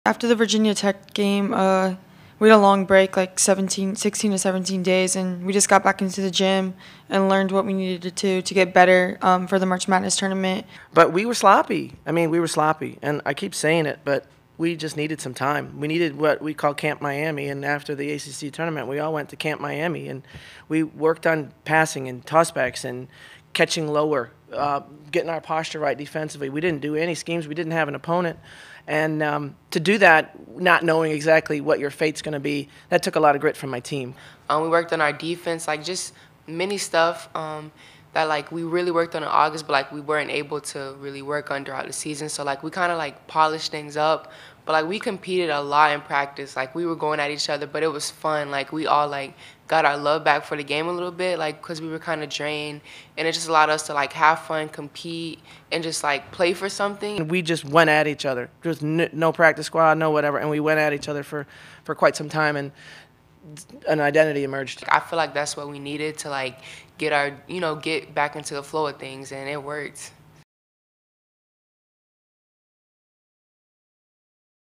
How Camp Miami Got The Hurricanes Women's Ready For The NCAA Tournament
After losing in the ACC Tournament to Virginia Tech, the Miami Hurricanes women's basketball used the time off before the NCAA Tournament to put in work.  Called "Camp Miami" by coach Katie Meier, listen to how the 'Canes were able to find their groove and get ready for their run to the Elite Eight.